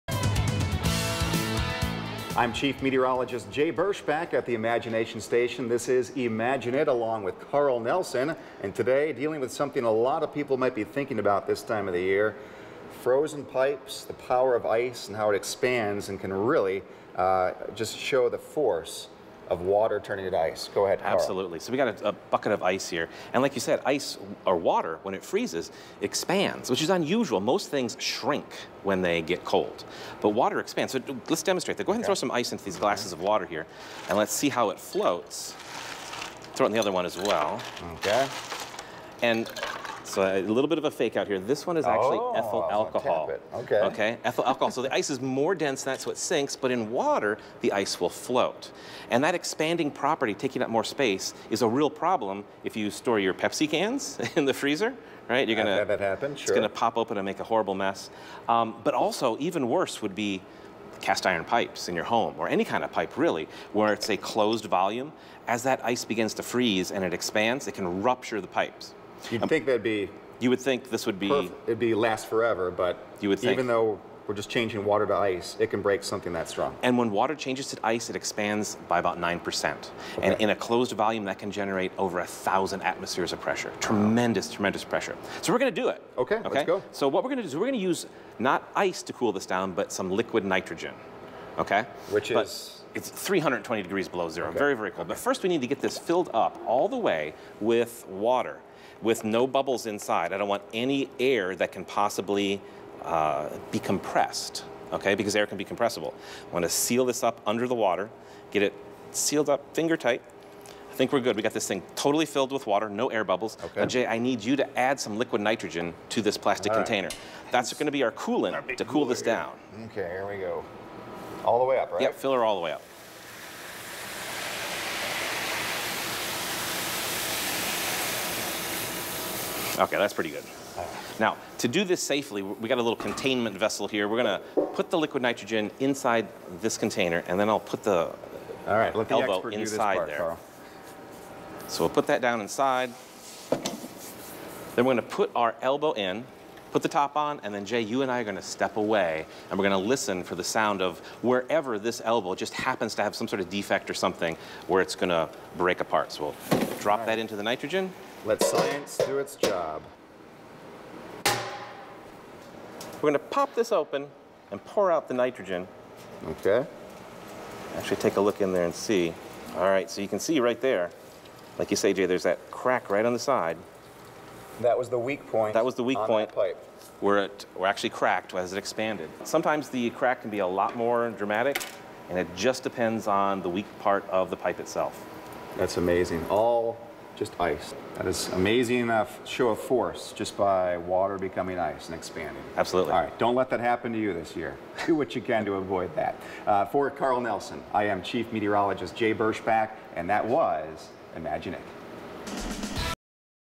Imagine It - Expanding ice bursts iron pipe
Chief Scientist Carl Nelson uses liquid nitrogen to freeze water inside a sealed pipe. The water freezes into ice, expands and ruptures the pipe.